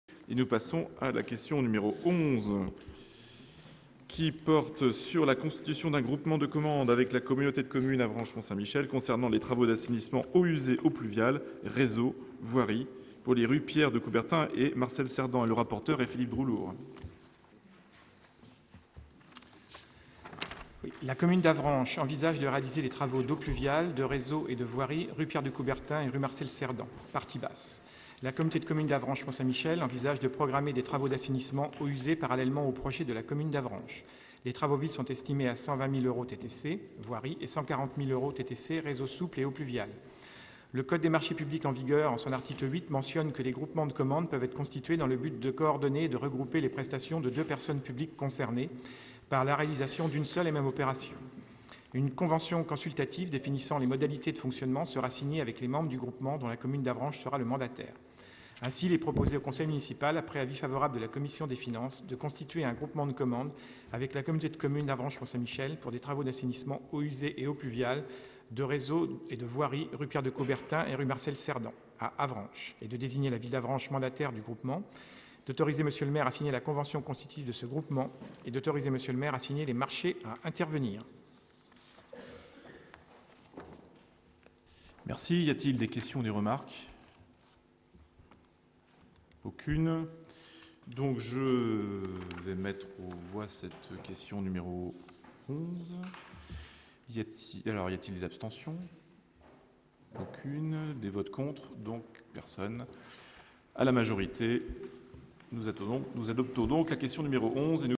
13 avril 2015 - Question 11
Constitution d'un groupement de commandes avec la communauté de communes Avranches - Mont Saint Michel concernant les travaux d'assainissement eaux usées - eaux pluviales - réseaux - voirie pour les rues Pierre de Coubertin et rue Marcel Cerdan.